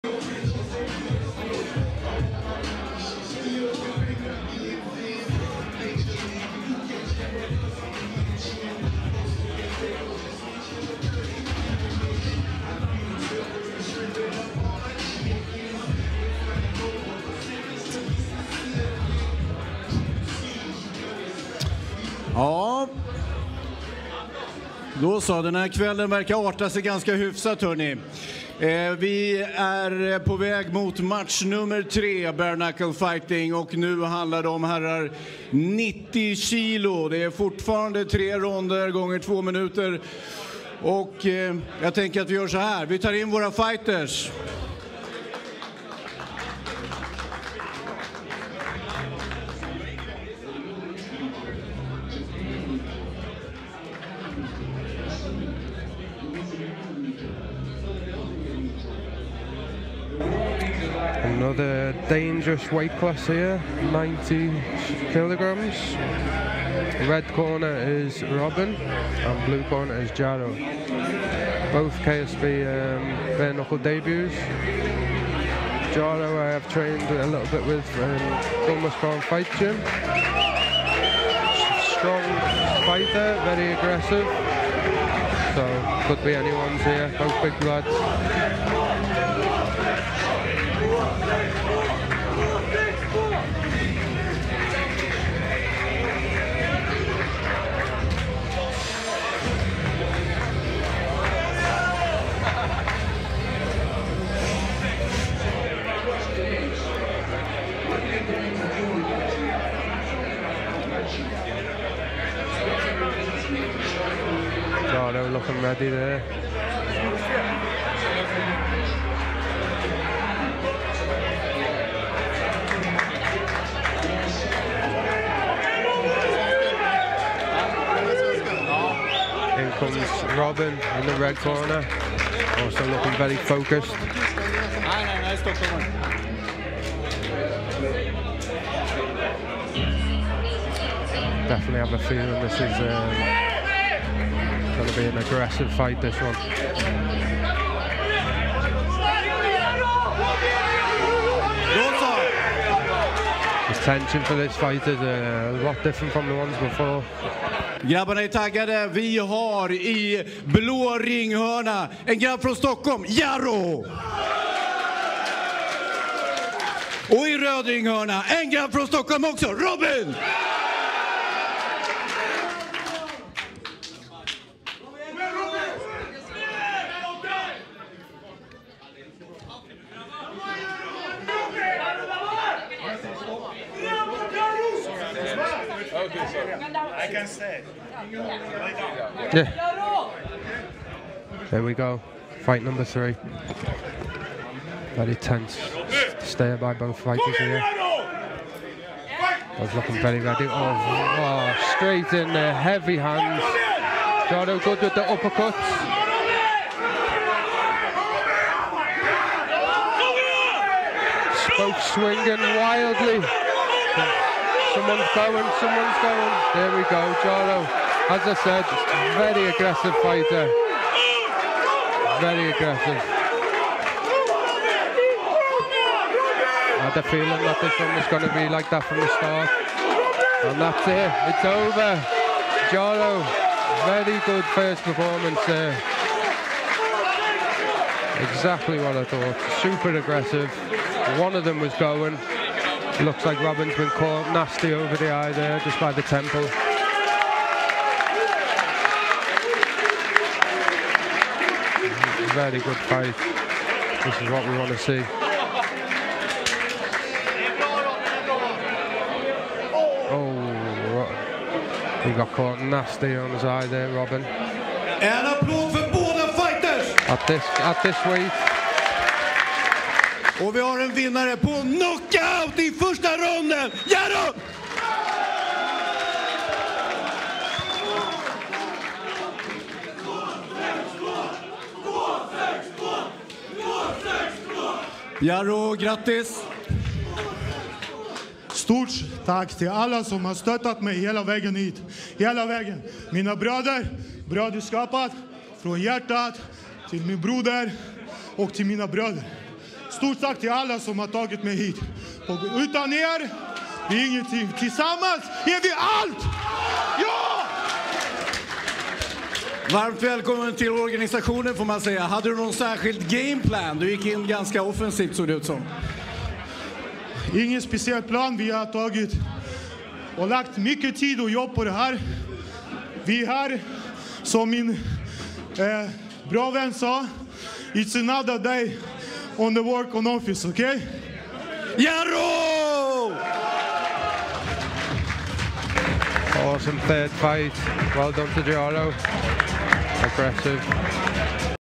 Watch FIGHT NIGHT 5 - Robin vs Yaro (Bare knuckle Boxing) BKB Bareknuckle fighting
👀🩸👊WATCH ALL FIGHT NIGHTS FOR FREE AT KSPPLAY.COM

Bare knuckle fighting Jaro wins by K.O first round